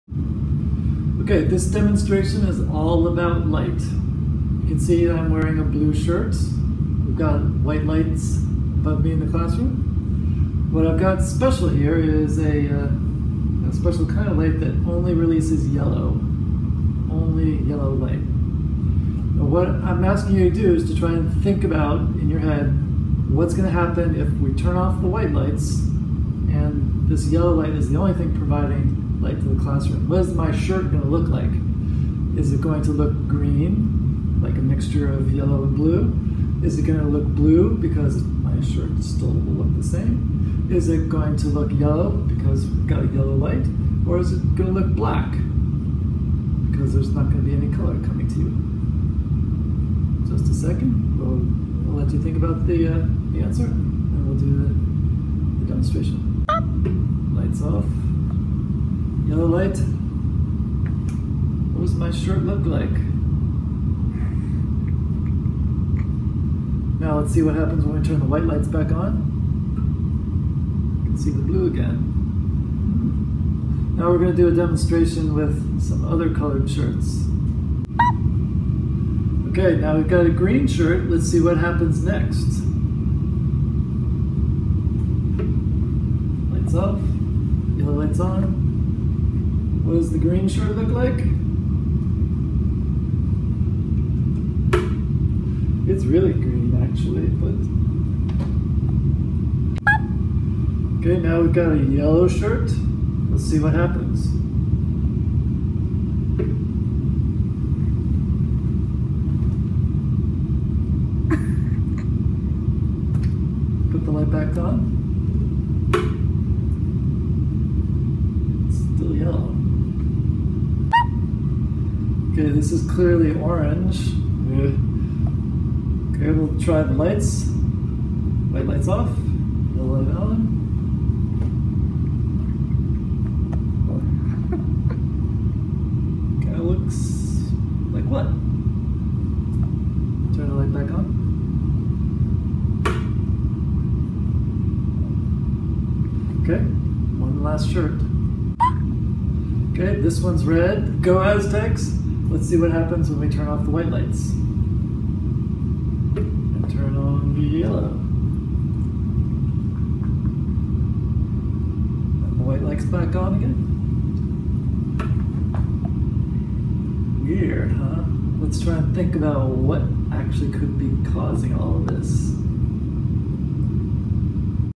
Monochromatic Light and Color Weirdness!
A demonstration of the effects of pure monochromatic light on the appearance of colorful clothes. This demo uses a low-pressure sodium street lamp to produce nearly pure yellow light, and contrasts the appearance of clothes in that yellow light and in white light.